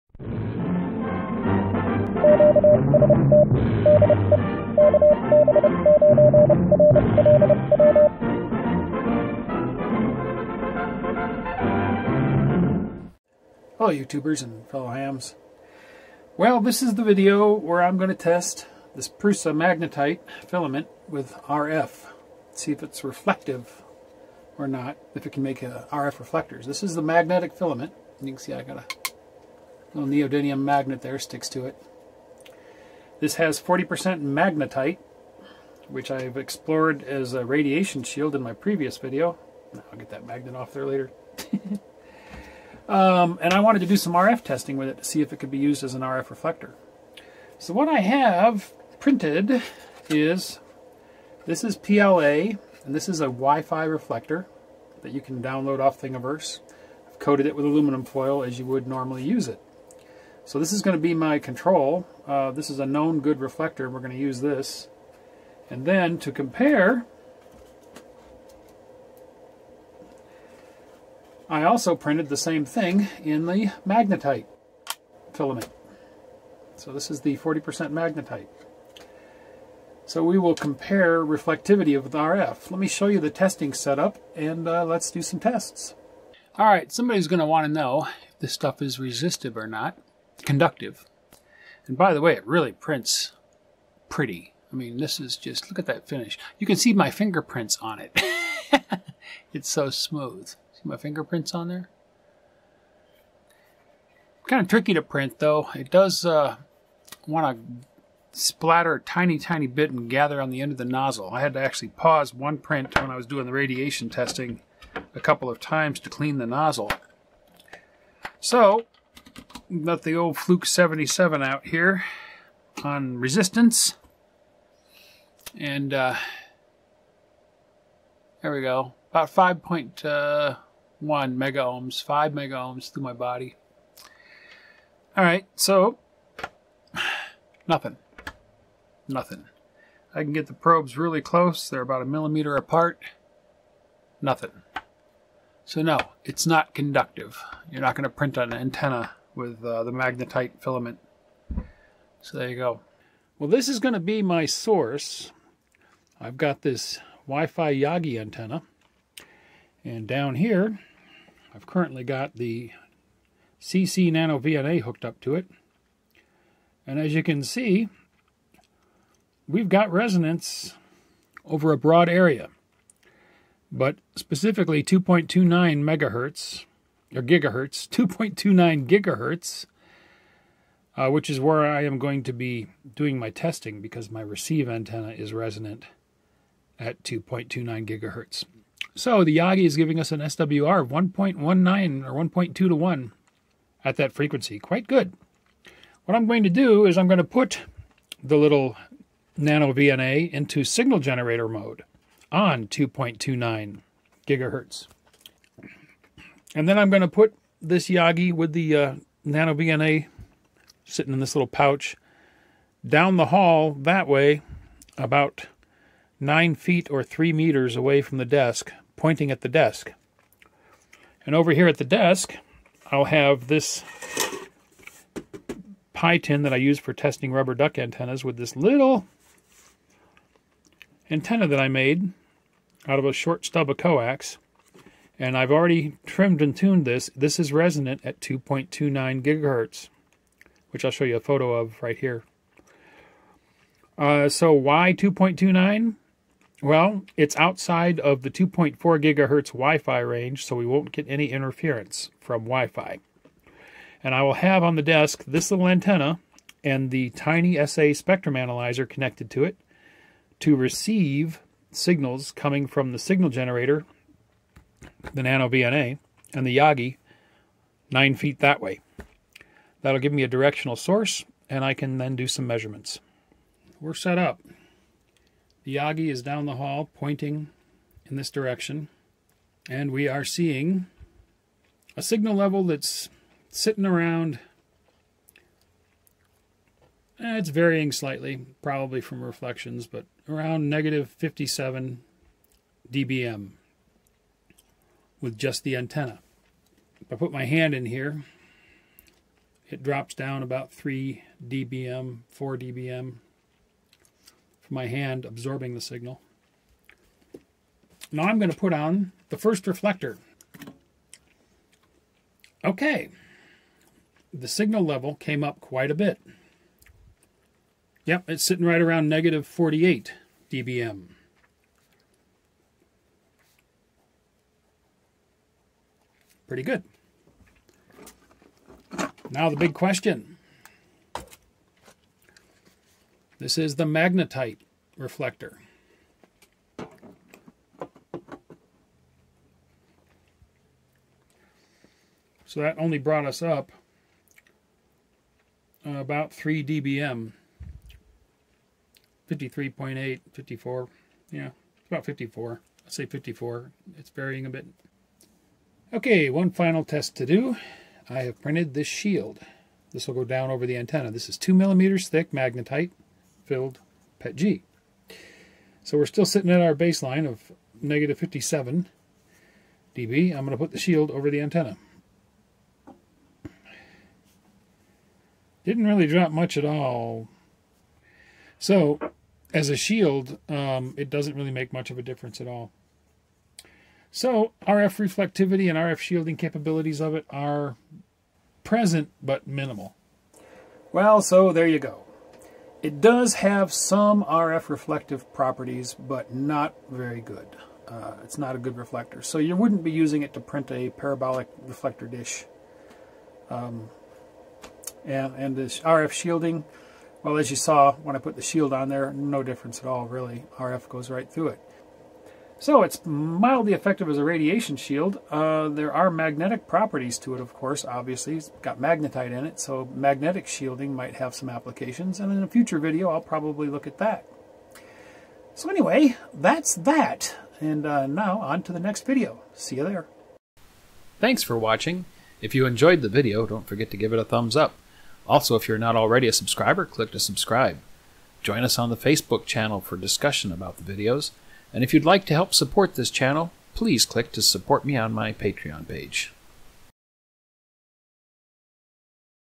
Ham radio & 3D printing - Prusament PETG magnetite filament, 3D printable RF reflector or shield?
I'm a little disappointed, but I kind of expected something like this.  It's only 40% magnetite, leaves a lot of plastic between the molecules for the RF to slip through.